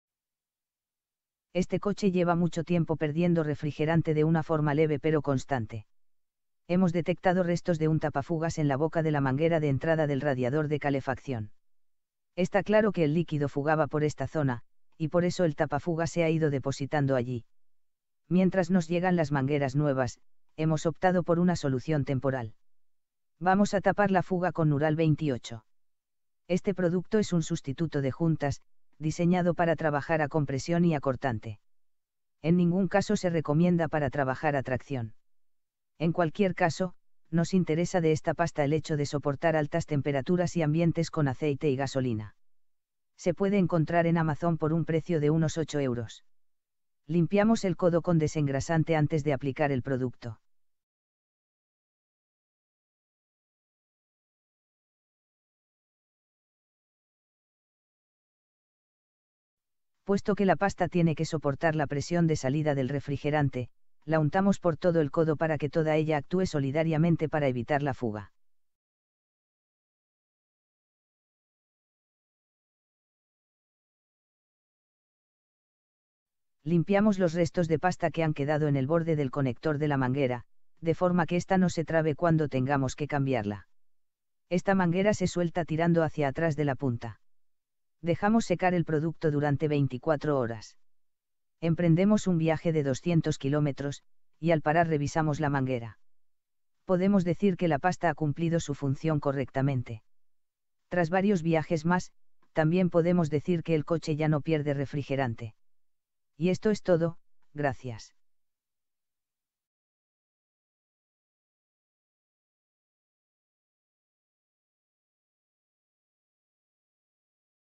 Como usar Nural 28 para tapar fugas en mangueras y manguitos del coche. Fuga de refrigerante. Averia
Este coche lleva mucho tiempo perdiendo refrigerante de una forma leve pero constante. Hemos detectado restos de un tapafugas en la boca de la manguera de entrada del radiador de calefacción. Está claro que el líquido fugaba por esta zona, y por eso el tapafugas se ha ido depositando allí.
Mientras nos llegan las mangueras nuevas, hemos optado por una solución temporal. Vamos a tapar la fuga con Nural 28. Este producto es un sustituto de juntas, diseñado para trabajar a compresión y a cortante. En ningún caso se recomienda para trabajar a tracción. En cualquier caso, nos interesa de esta pasta el hecho de soportar altas temperaturas y ambientes con aceite y gasolina. Se puede encontrar en Amazon por un precio de unos 8 euros.

Limpiamos el codo con desengrasante antes de aplicar el producto. Puesto que la pasta tiene que soportar la presión de salida del refrigerante, la untamos por todo el codo para que toda ella actúe solidariamente para evitar la fuga.

Limpiamos los restos de pasta que han quedado en el borde del conector de la manguera, de forma que esta no se trabe cuando tengamos que cambiarla. Esta manguera se suelta tirando hacia atrás de la punta.

Dejamos secar el producto durante 24 horas. Emprendemos un viaje de 200 kilómetros, y al parar revisamos la manguera. Podemos decir que la pasta ha cumplido su función correctamente. Tras varios viajes más, también podemos decir que el coche ya no pierde refrigerante.

Y esto es todo, gracias.